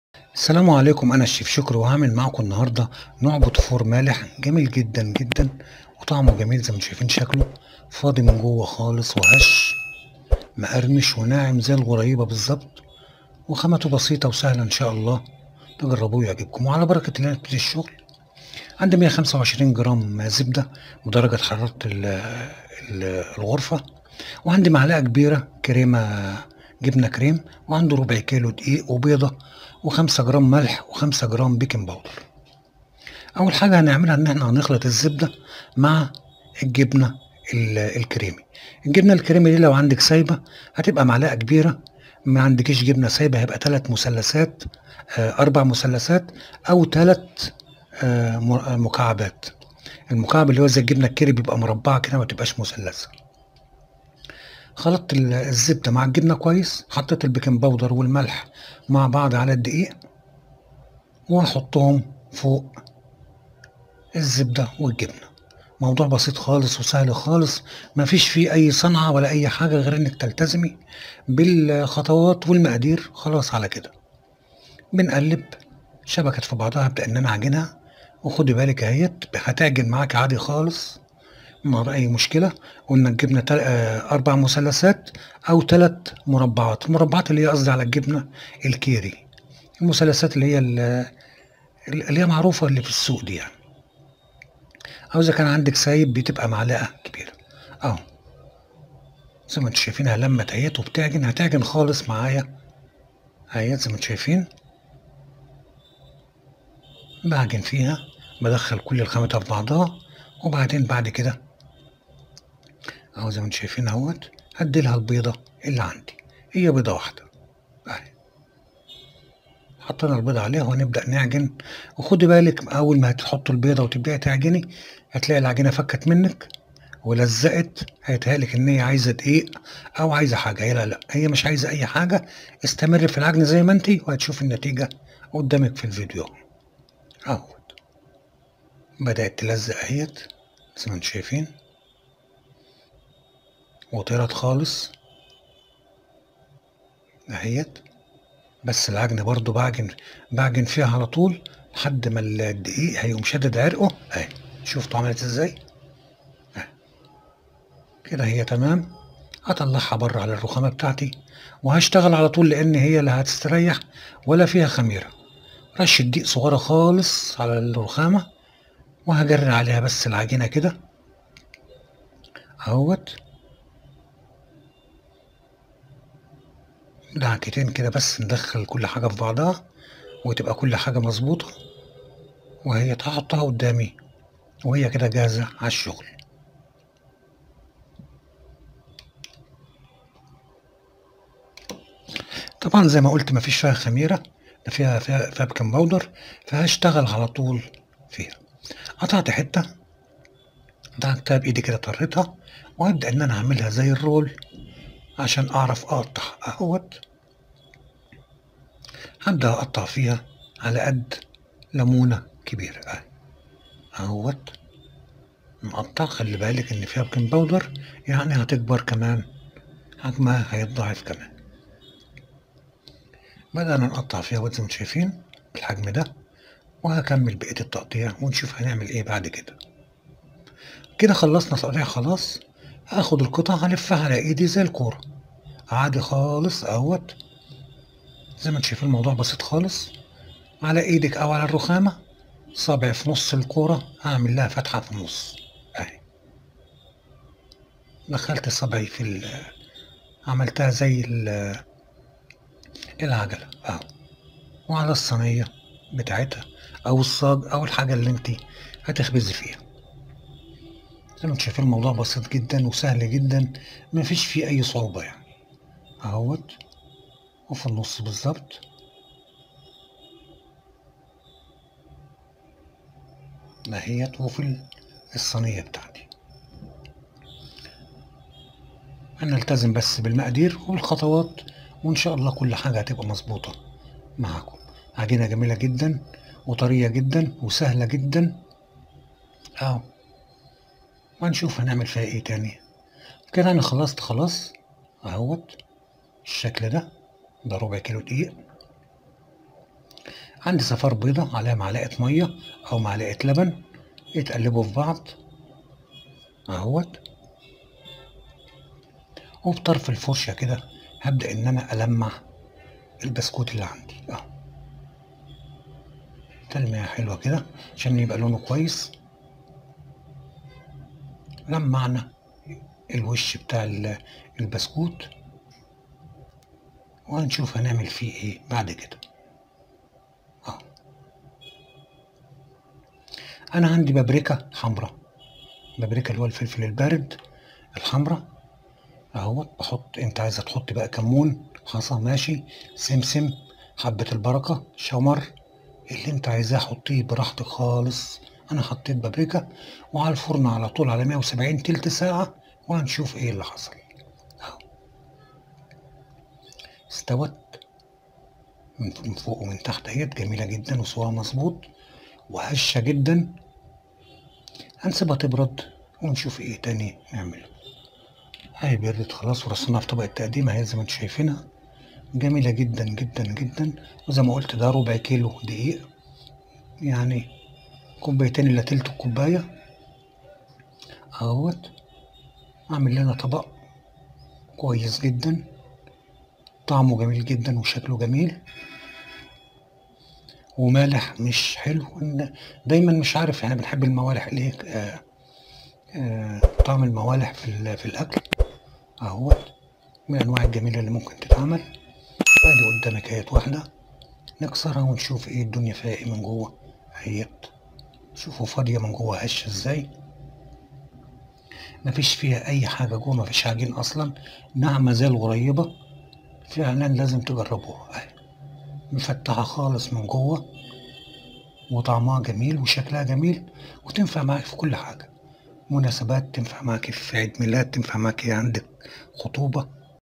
عمل بتيفور مالح سهل و سريع و بسيط مع الشيف شكري
عمل بتيفور مالح سهل و سريع و بسيط تعتبر الموالح تشكيلة من المخبوزات، وهي من الأطعمة التي يمكن تقديمها في أيّ مكان ووقت سواء على وجبة العشاء أو الغذاء أو الفطور، حيث تعدّ وجبة غذائيّة متكاملة بجانب الشوربات والسلطات والأطباق الأخرى، وتمتاز بأنها خفيفة على المعدة ومشبعة في نفس الوقت، ويمكن تحضير تشكيلة واسعة منها بكل سهولة وسرعة،
تشكيله من المعجنات السواريه التي تصلح الي جميع المناسبات والحفلات  واعياد الميلاد

لو عجبك الفيديو اشتزك في القناه و فعل زر الجرس ليصلك كل جديد

بتيفور مالح
40 جرام جبنه كريم      معلقه كبيره
125 جرام زبده             نصف كوب
250 جرام دقيق           2 كوب الا ثلث
5 جرام بكنج بودر         نصف معلقه صغيره
5 جرام ملح                  نصف معلقه صغيره